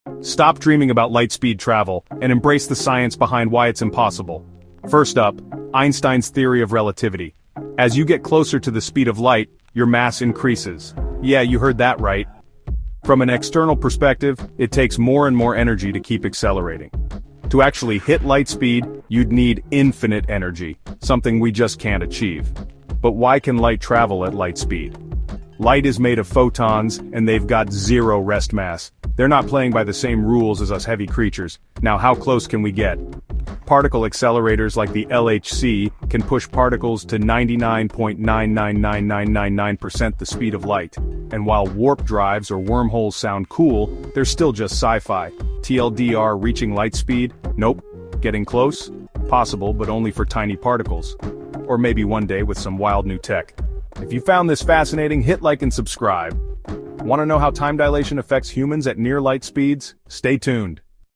Can we ever reach light speed? 🚀
Can we ever reach light speed? 🚀 In this thought-provoking video, we dive into the fascinating world of physics to explore why traveling at the speed of light remains impossible with our current understanding. Discover how Einstein’s Theory of Relativity reveals the challenges of mass and energy as we approach light speed. We’ll also discuss the unique characteristics of photons and what advancements in technology, like particle accelerators, can achieve. Join us as we ponder the potential of warp drives and wormholes for future travel beyond our current limits.